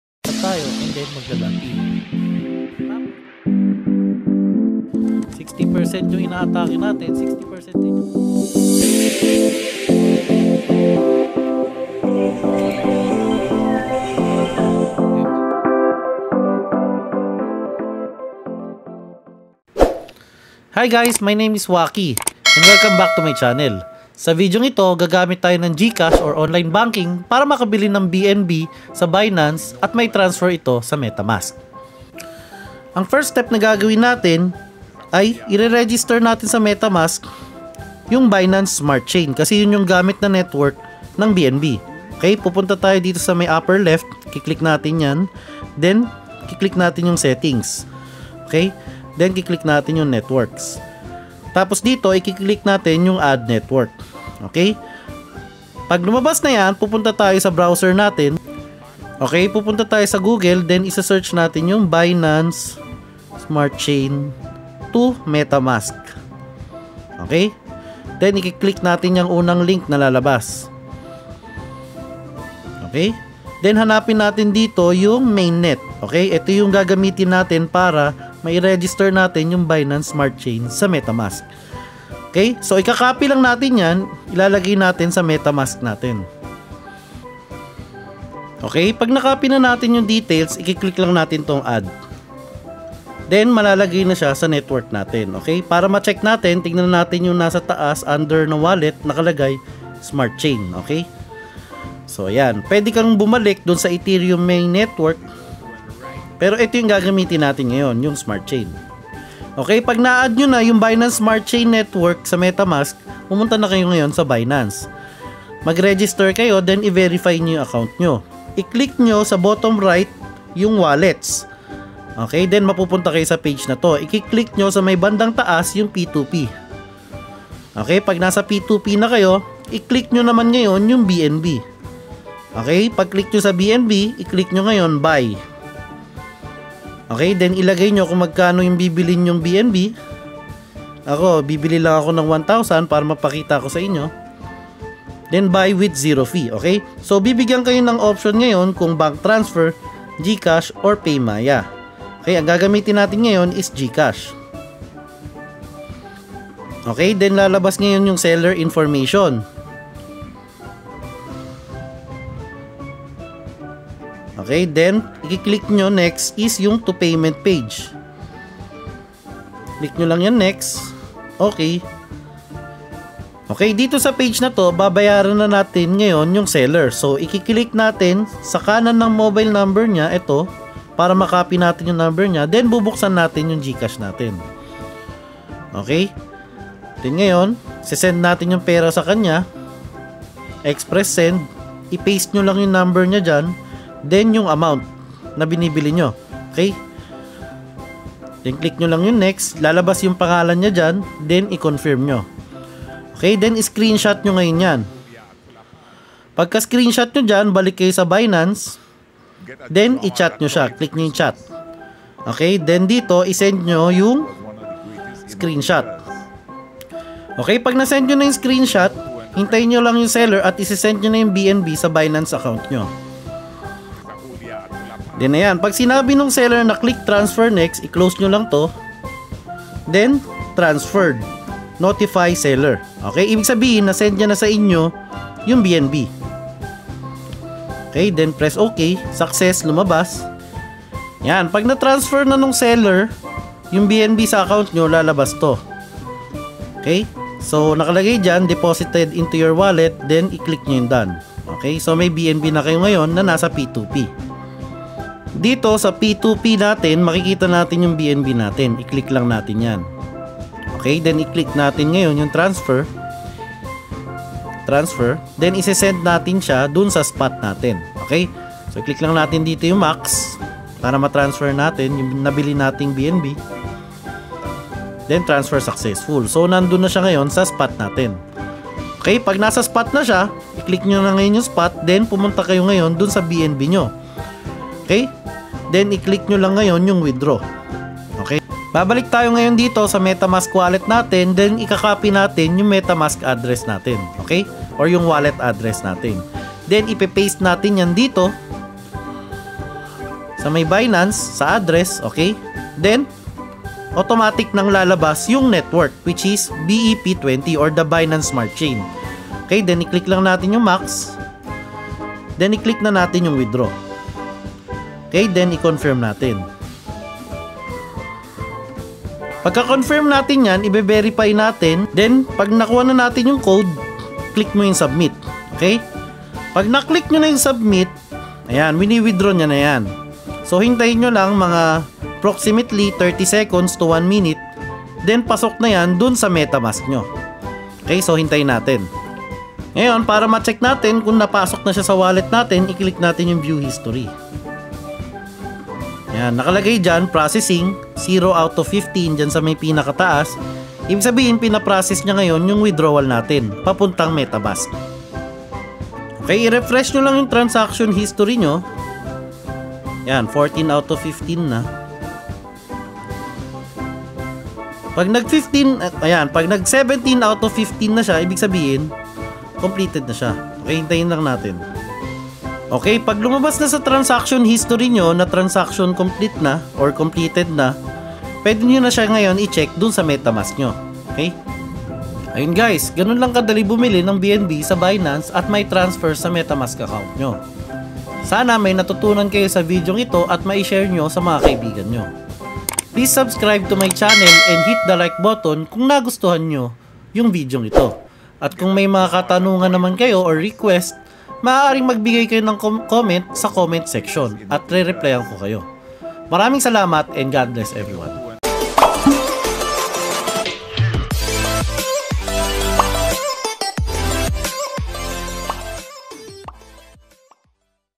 BINANCE TO METAMASK: How to buy BNB using Gcash to Binance to Metamask (MOBILE EDITION)
BINANCE TO METAMASK: How to buy BNB using Gcash or Online Banking to Binance to Metamask (Binance Smart Chain Network - BSC) - MOBILE EDITION